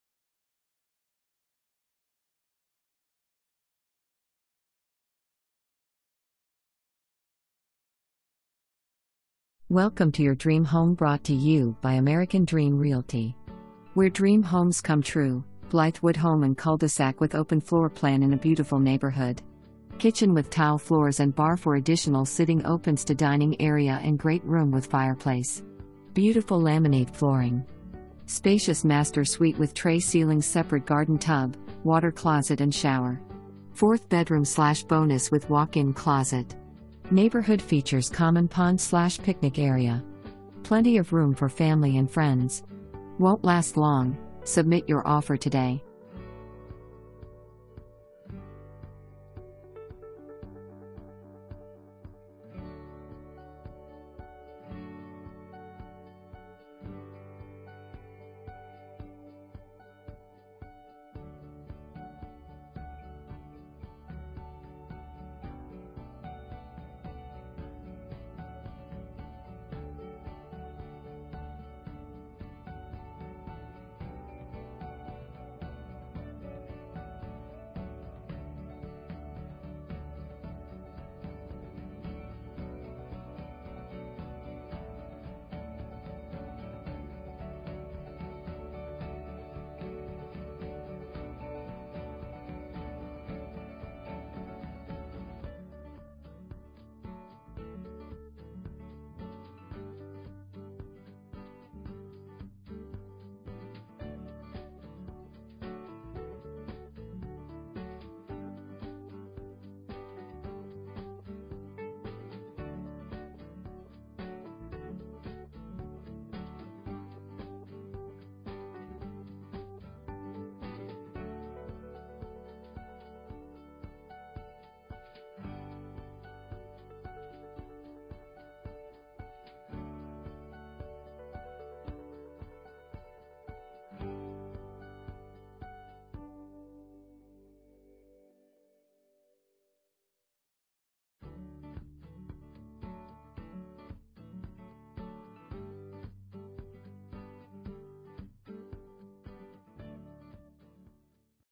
Blythewood home in cul-de-sac with open floor plan in a beautiful neighborhood.
182 Summer Pines Drive
Blythewood, SC 29016

Blythewood home in cul-de-sac with open floor plan in a beautiful neighborhood. Kitchen with tile floors and bar for additional sitting opens to dining area & great room with fireplace. Beautiful laminate flooring. Spacious master suite with trey ceilings separate garden tub, water closet & shower. 4th bedroom/bonus with walk-in closet. Neighborhood features common pond/picnic area. Plenty of room for family and friends. Won't last long, submit your offer today!

Pam Clamp Fran Smith
American Dream Realty